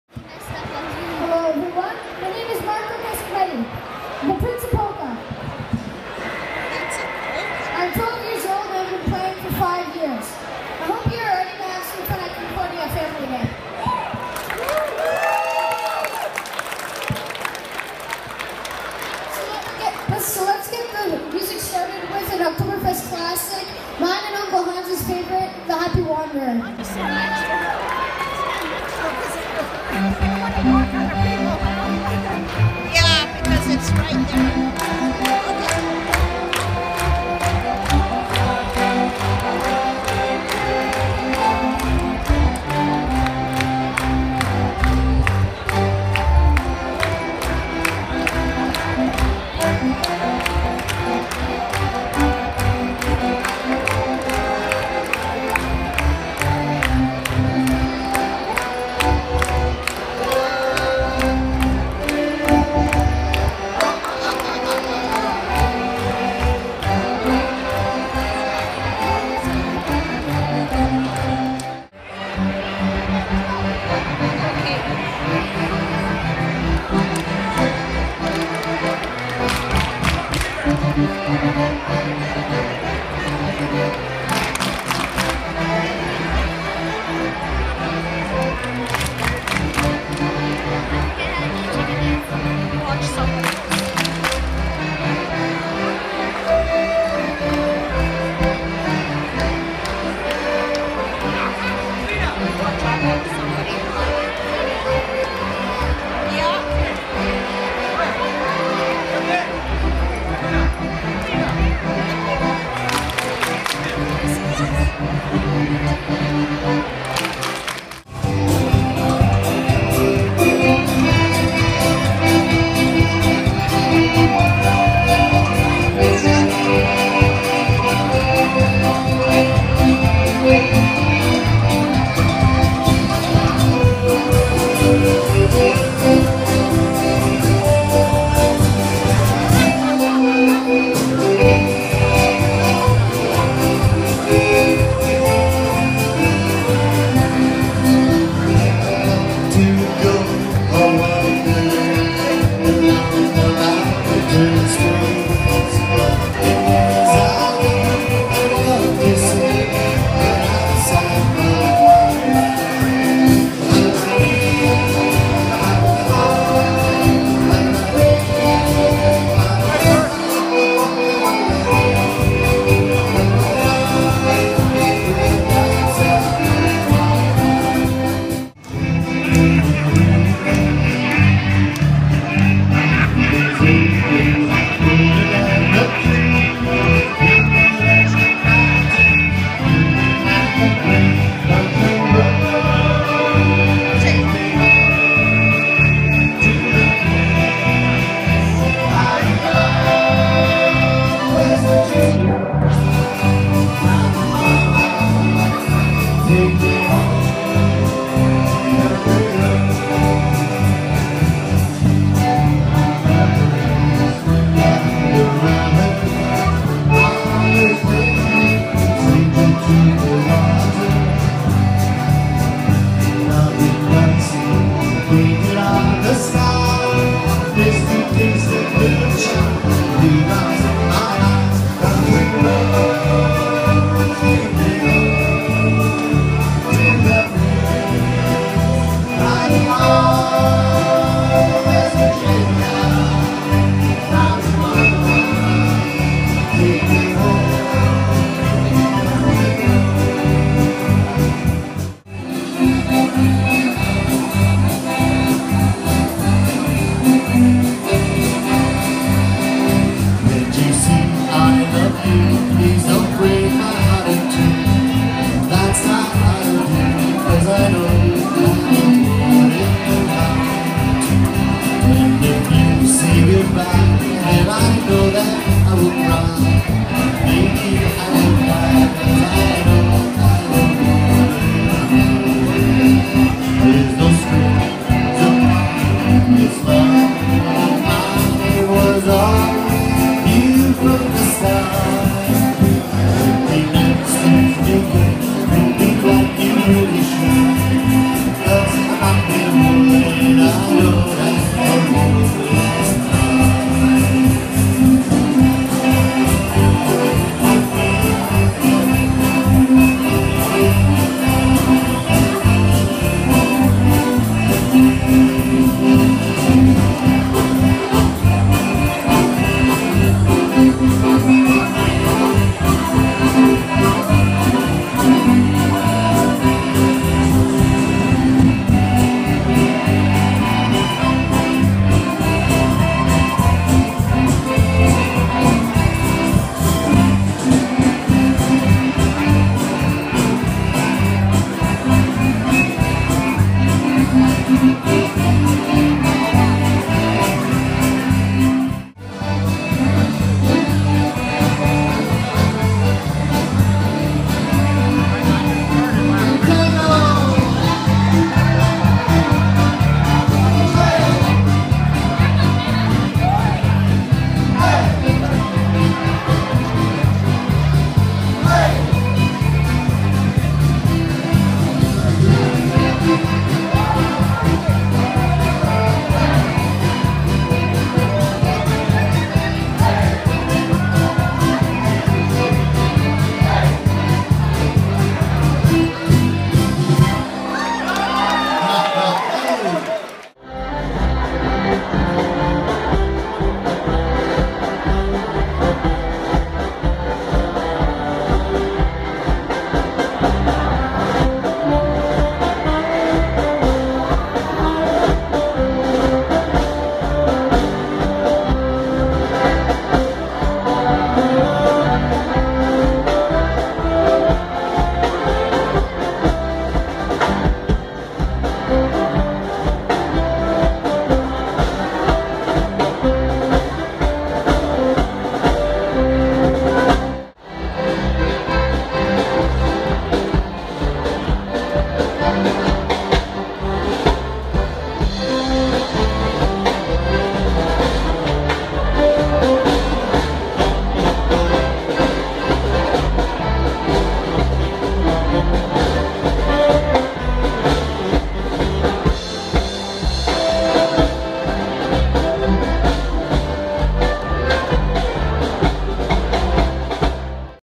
12 Year Old Marko, "Prince of Polka" Does Oktoberfest 2016
12 year old Marko Kecskemeti, Prince of Polka, is wowing the audience again at Oktoberfest. He is gaining much experience as guest artist with many different bands. Notice the twosie he gets with a pretty young lady at the end of this clip.